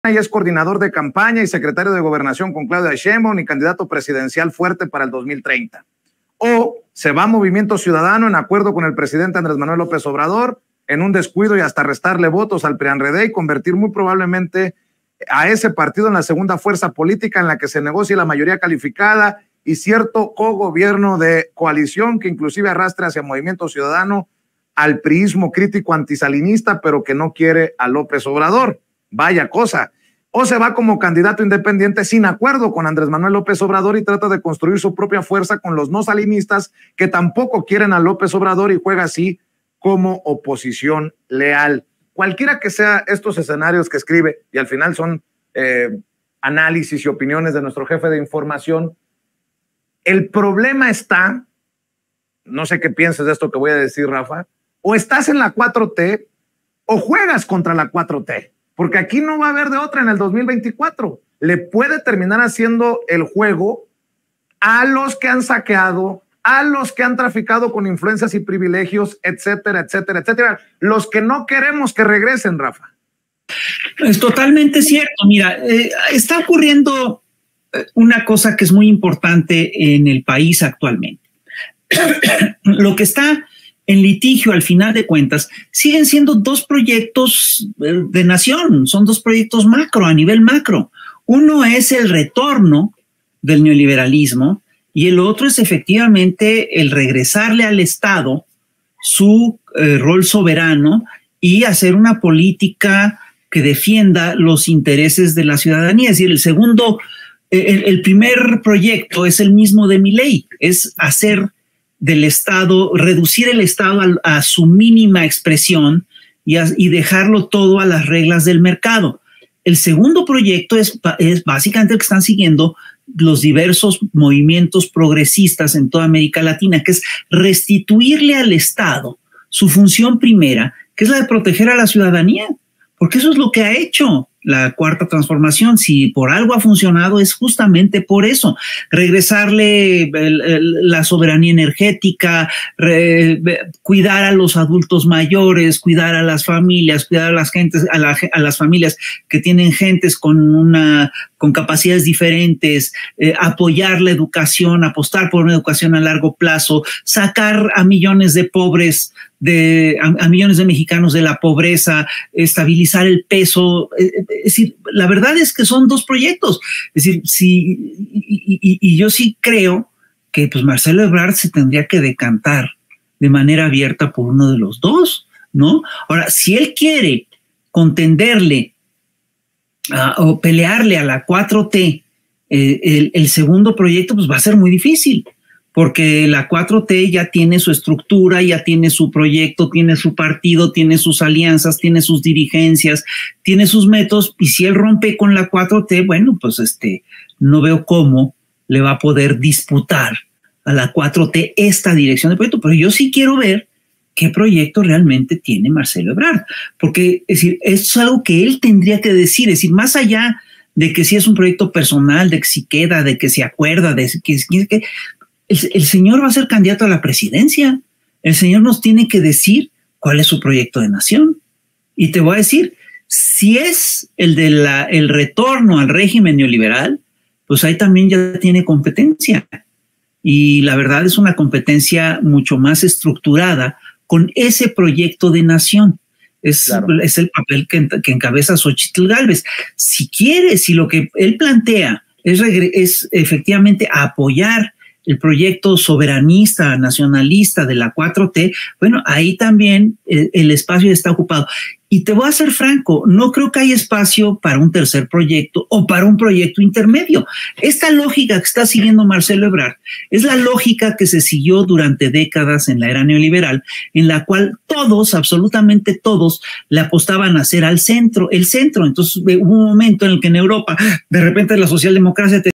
FISGÓN DICE QUE SI EBRARD LE APUESTA A PELEAR CONTRA LA 4T, LA TIENE MÁS QUE DIFÍCIL
Sobre los escenarios posibles que esperan a Marcelo Ebrard, el caricaturista y periodista Rafael Barajas Fisgón Monero sostuvo que lo que lo que está en litigio a final de cuentas son dos proyectos de nación: Uno, el retorno del neoliberalismo, y el otro regresar al estado su rol soberano, pero que si Ebrard quiere pelear contra la 4T la tiene más que difícil